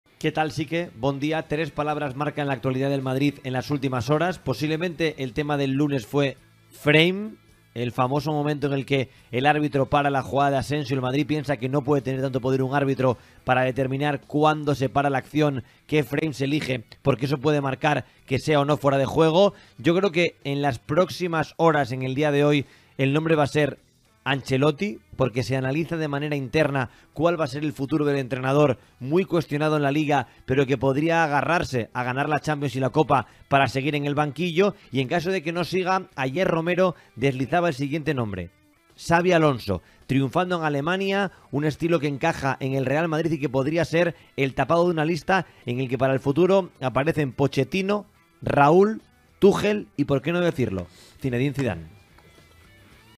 REAL MADRID | Los CANDIDATOS al BANQUILLO: TUCHEL, ZIDANE, POCHETTINO, XABI ALONSO...| DIARIO AS
Antón Meana desgranó los entrenadores que podrían ocupar el puesto de Ancelotti de cara a la temporada que viene.